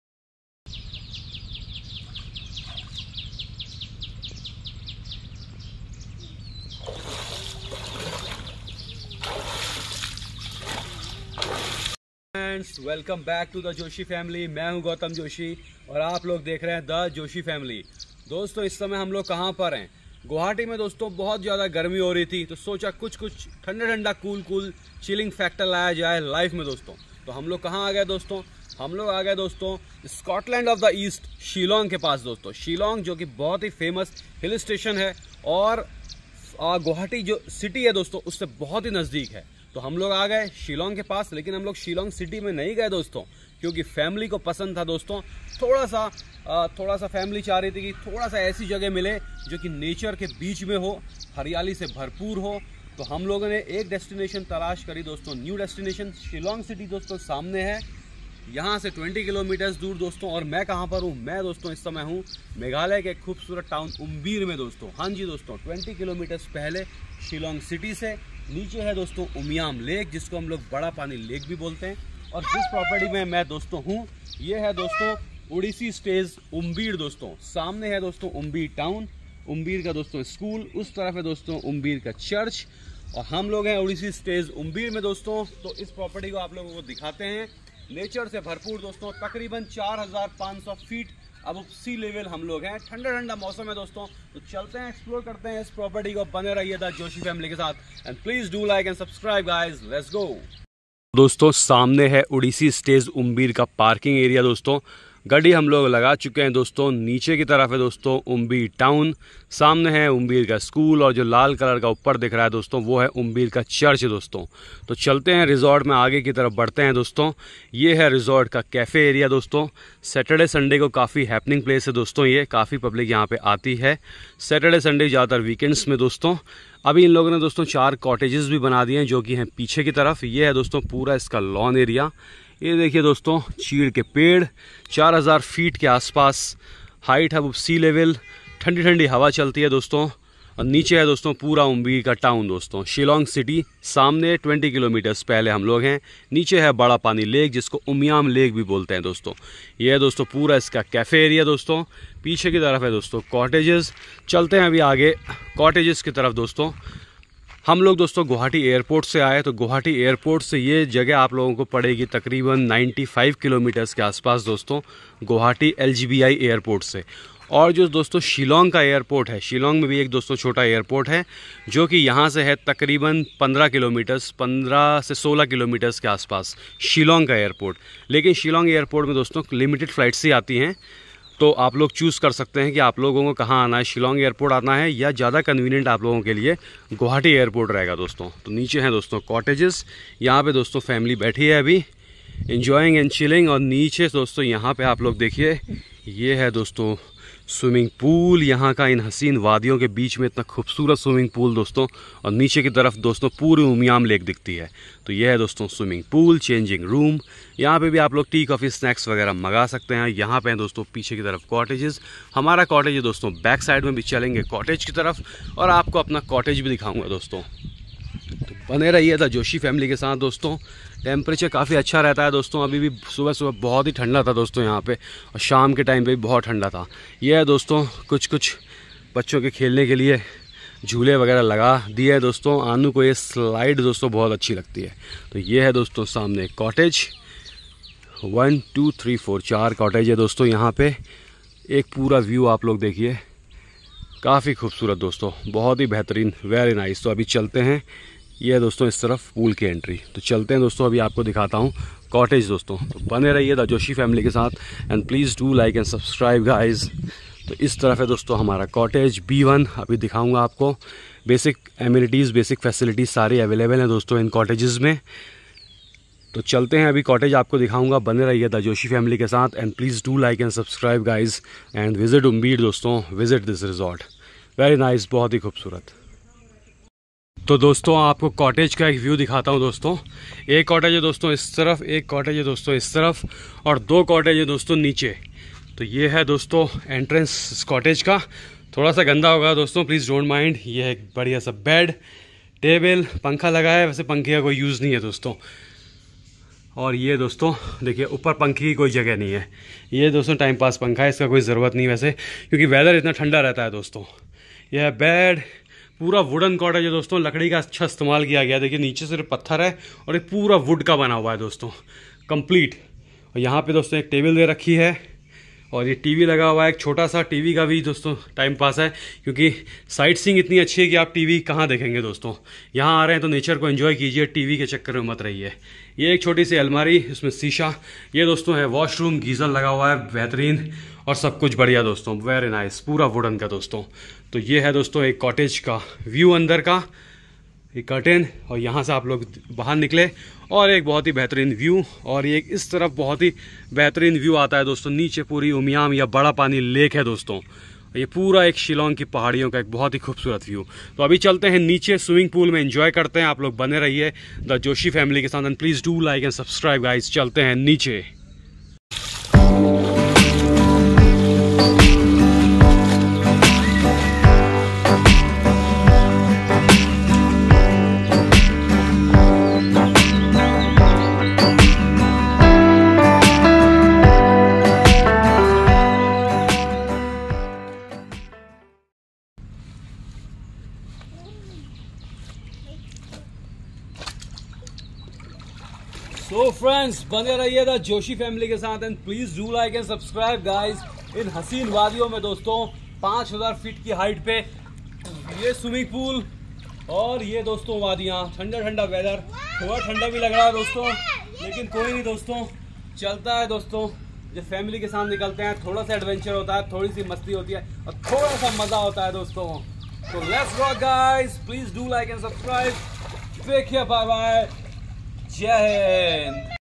Odyssey Stays Umbir Meghalaya I Best Resort Near Shillong With Swimming pool I Luxury Stay In Budget
Odyssey stays umbir near umiam lake barapani shillong Meghalaya.Hello friends so in this vlog we will show u a great property in umbir town Meghalaya.Its very close to Shillong city and also shillong Airport.Also Guwahati lgbi airport is only 95 km from there.The name of the Resort is Odyssey stays umbir.It is a great distination if u want to relax in between Nature.Climate there is very pleasant throughout the year. Shillong city from Odyssey stays is just 20 km and umiam barapani lake is just 3 km.